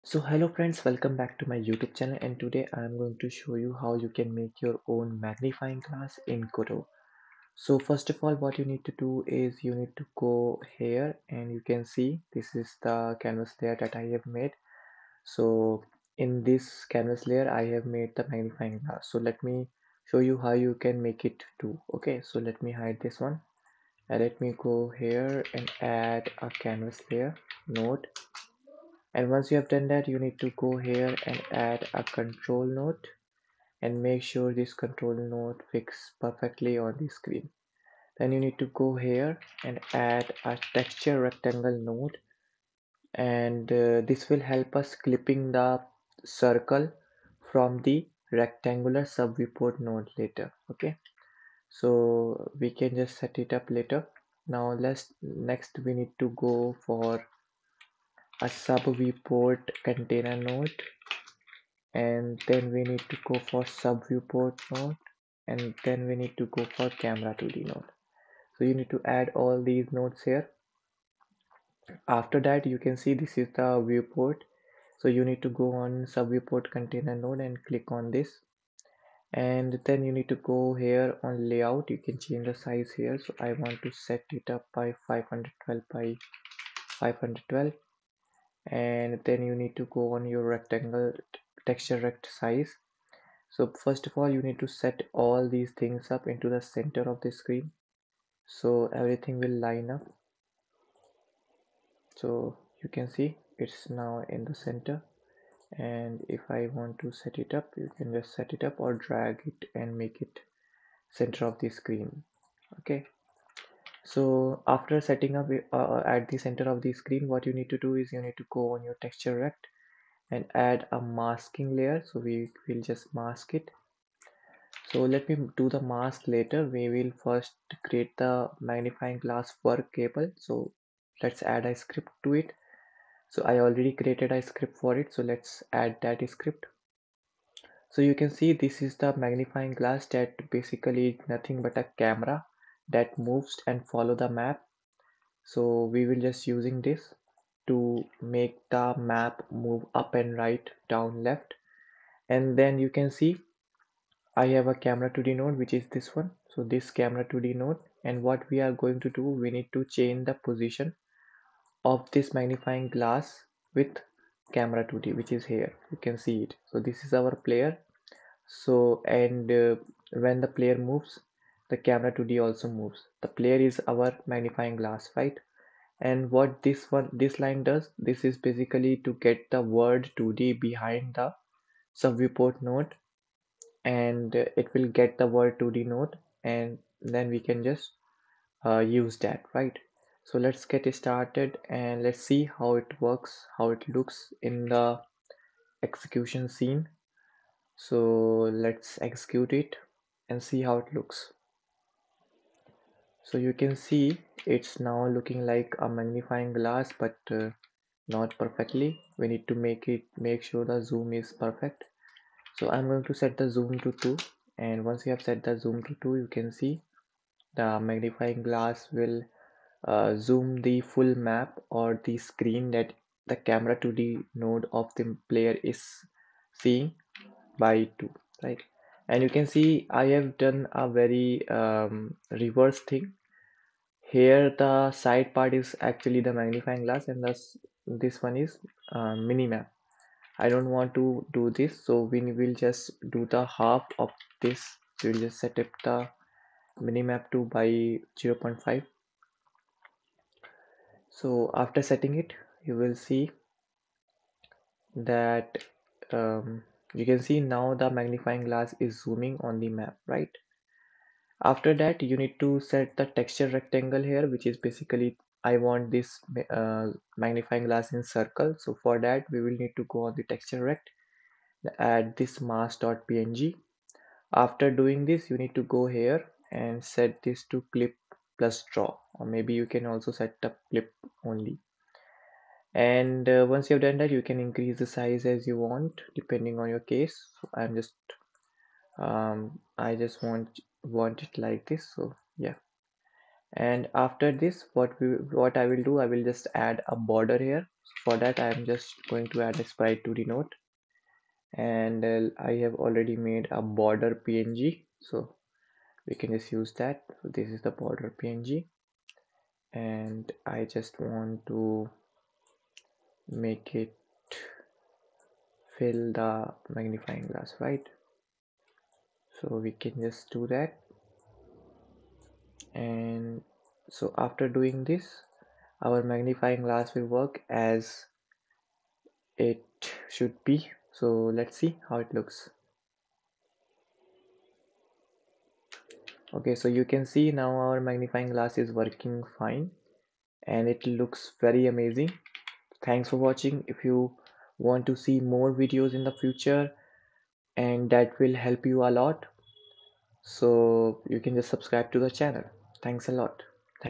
How to make magnifying glass in Godot 4+ | Magnifying Glass tutorial 2d
Learn how to create a 2D magnifying glass effect in Godot 4+ using SubViewport! In this tutorial, we’ll use SubViewport and textures to create a smooth and efficient zoom effect, perfect for puzzle games, hidden object games, or any project that needs a magnification mechanic.

🔥 What You'll Learn:
✅ Setting up a SubViewport for magnification
✅ Using textures to create a zoom effect
✅ Applying the effect dynamically in your game